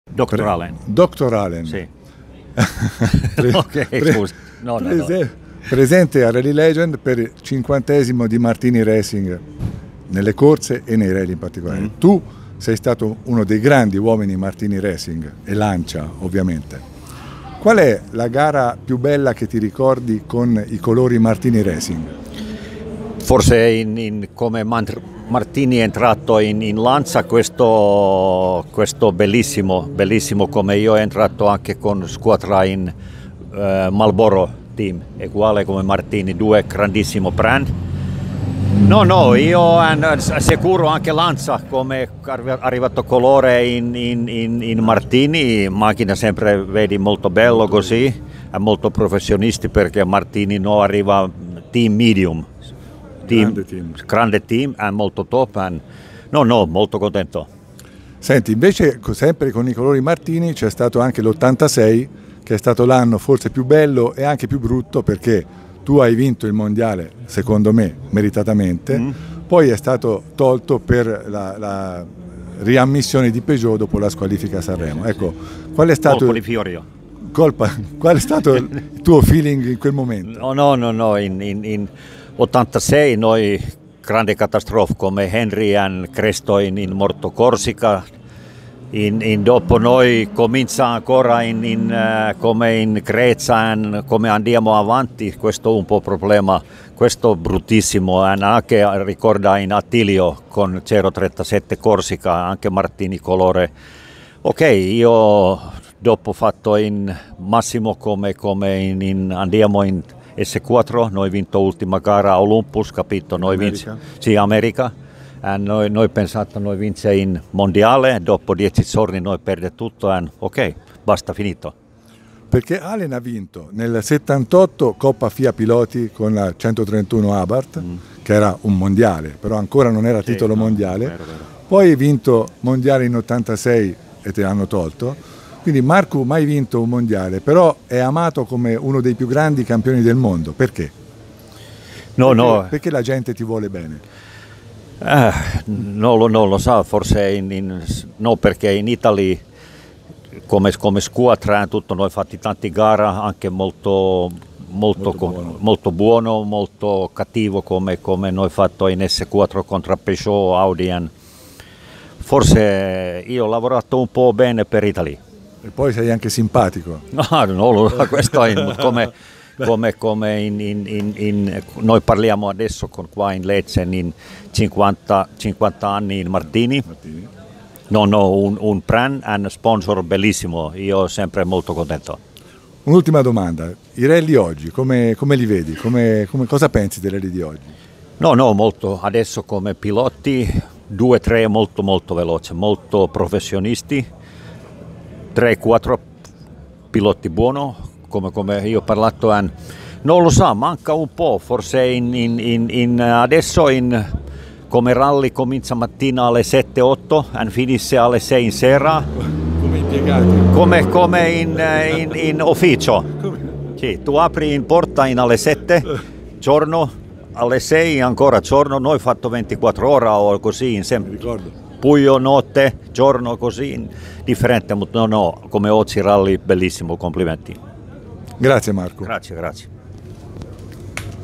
INTERVISTA MARKKU ALEN IN OCCASIONE DEL 50° MARTINI RACING A RALLYLEGEND 2018
Il grande Markku Alen racconta, nel suo italiano-"alenese", i suoi ricordi nel team Lancia Martini Racing